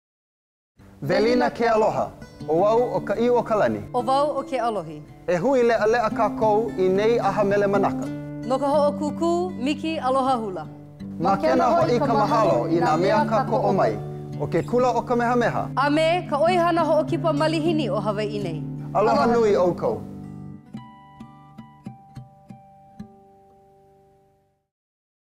Merrie Monarch Festival - Miss Aloha Hula, Hula Kahiko, Hula ʻAuana
Join us for our sixth year of Hawaiian Language Secondary Audio Program, a feature of our KFVE Television Broadcast, beginning with the Miss Aloha Hula program, Thursday April 25th.

Supported  by a sponsorship from Kamehameha Schools and The Hawaiʻi Tourism Authority.

For more information on how to tune in for the SAP feature, visit the Merrie Monarch Festival website - merriemonarch.com